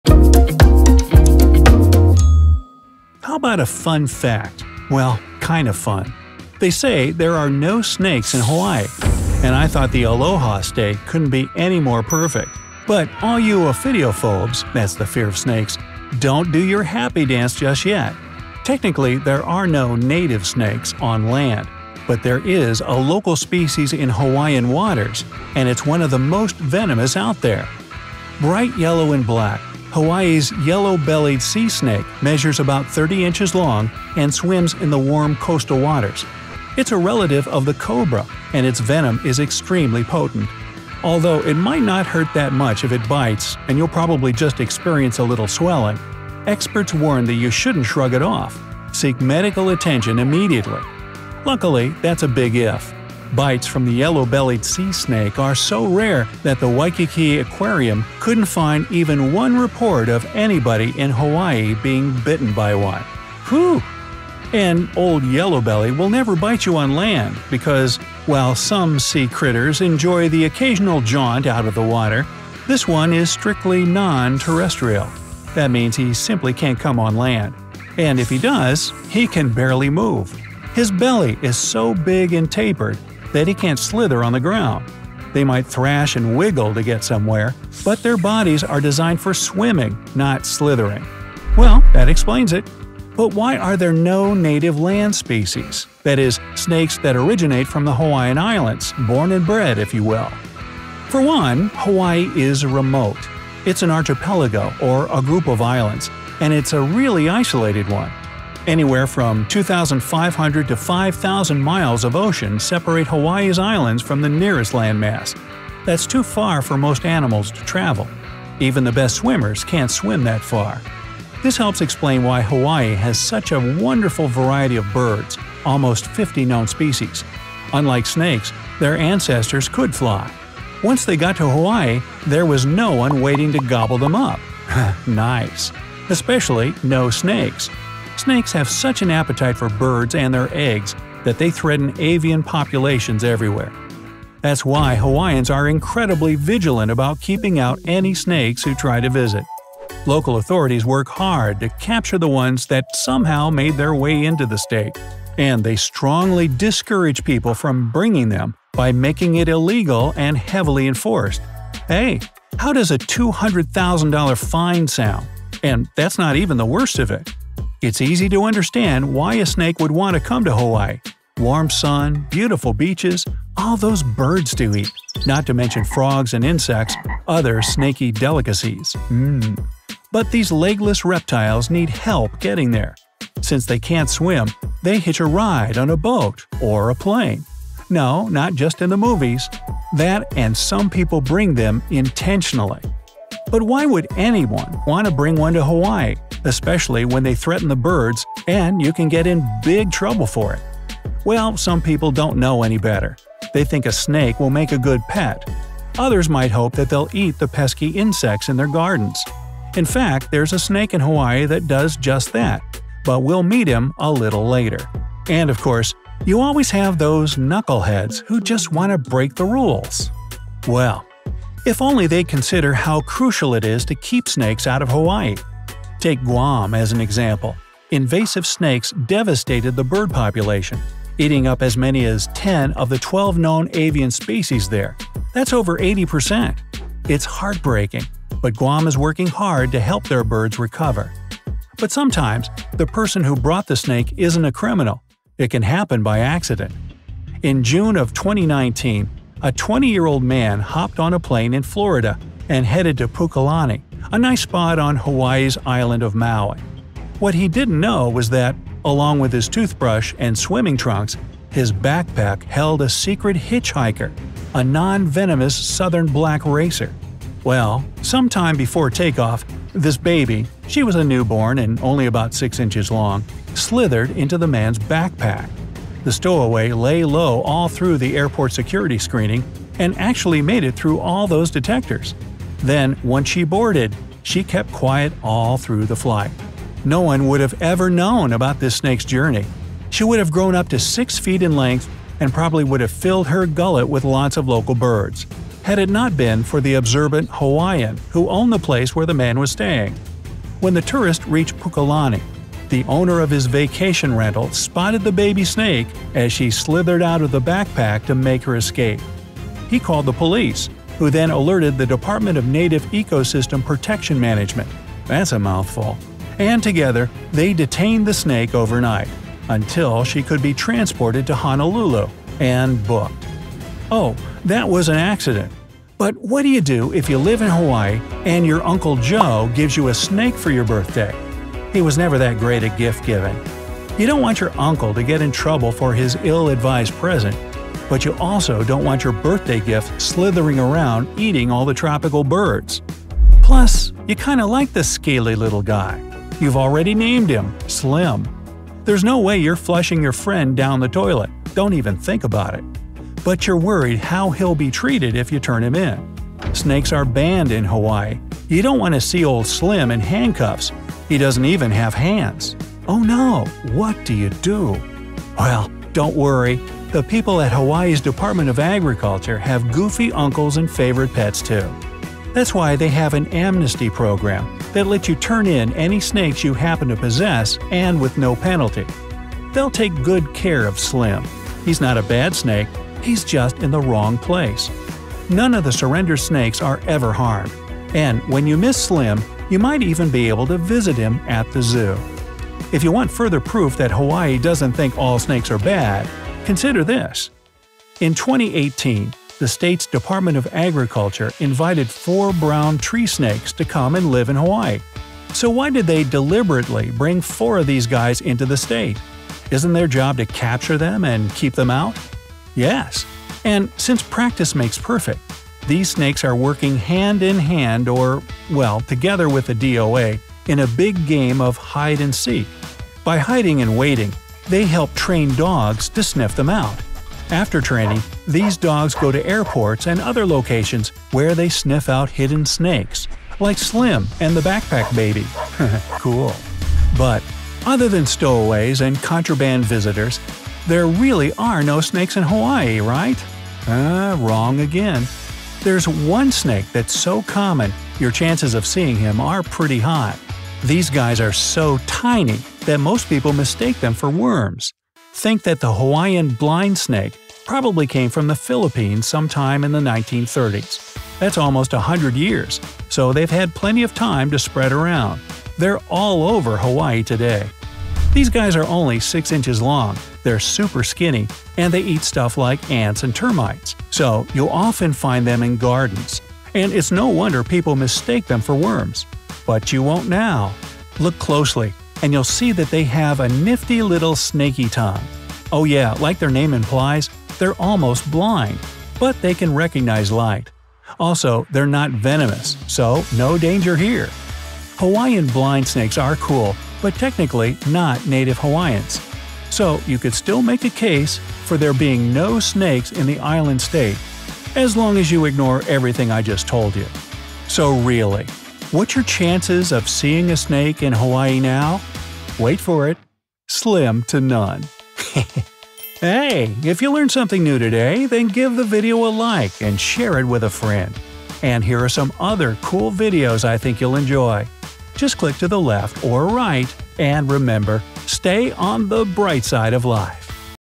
Are There Snakes in Hawaii? Turns Out Yes But Special
Are there snakes in Hawaii? Technically, there are no native snakes… on land. But there are a local species in Hawaiian waters.

Bright yellow and black, Hawaii’s Yellow-Bellied Sea Snake measures about 30 inches long and swims in the warm coastal waters. It's strictly non-terrestrial.

TIMESTAMPS:
Extremely venomous Yellow-Bellied Sea Snake 0:28
Why are there no native land species? 1:33
$200,000 fine (and that’s not the worst of it) 2:30
Devasted bird population 3:42
Secret hitchhiker 4:01
What if somebody gives you a snake for your birthday? 5:23
Not all snakes there are bad! 6:32
Is it a snake or a worm? 7:16

SUMMARY:
- Bites from the Yellow-Bellied Sea Snake are so rare that the Waikiki Aquarium couldn’t find even one report of anybody in Hawaii being bitten by one.
- Anywhere from 2,500 to 5,000 miles of ocean separate Hawaii’s islands from the nearest landmass. That’s too far for most animals to travel.
- Hawaiians are incredibly vigilant about keeping out any snakes who try to visit. Local authorities work hard to capture the ones that’ve somehow made their way into the state.
- Take Guam as an example – invasive snakes devasted the bird population, eating up as many as 10 of the 12 known avian species there.
- But sometimes the person who brought the snake isn’t a criminal – it can happen by accident.
- There is an amnesty program that lets you turn in any snakes you happen to possess and with no penalty!
- In 2018, the state’s Department of Agriculture invited 4 brown tree snakes to come and live in Hawaii.
- These snakes are working hand in hand or … well … together with the DoA in a big game of “Hide and Seek.” By hiding and waiting, they help train dogs to sniff them out.
- Also, there’s one snake that’s so common, your chances of seeing him are pretty high. These guys are so tiny that most people mistake them for worms!
- They’re almost blind, but they can recognize light. Also, they’re not venomous, so no danger here!